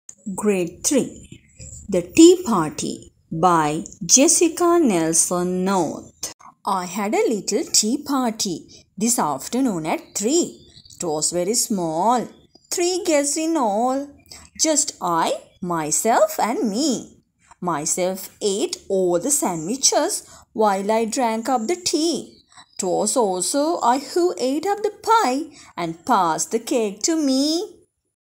Grade-03 poem "The tea party by Jessica Nelson North"
English day poem
Adult learners too can go through this video and can practice the accent